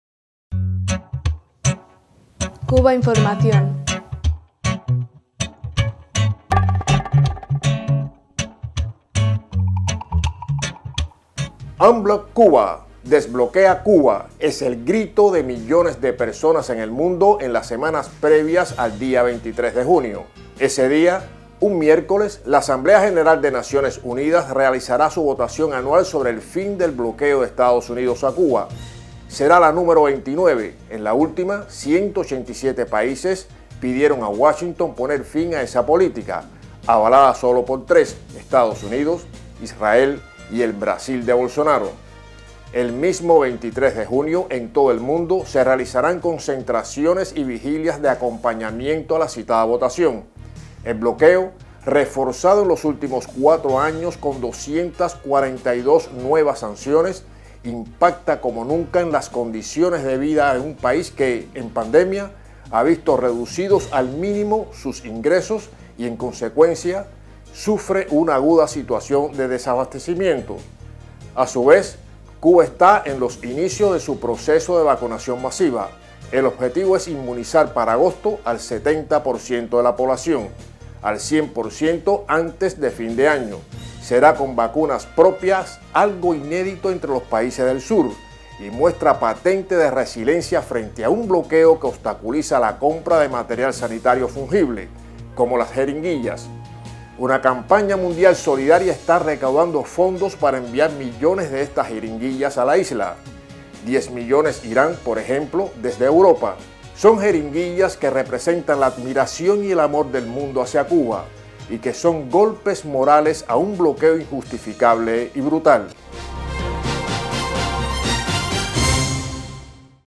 Jeringuillas de amor por Cuba, aguijones contra el bloqueo
Unblock Cuba!, ¡Desbloquea Cuba!, es el grito de millones de personas en el mundo, en las semanas previas al día 23 de junio.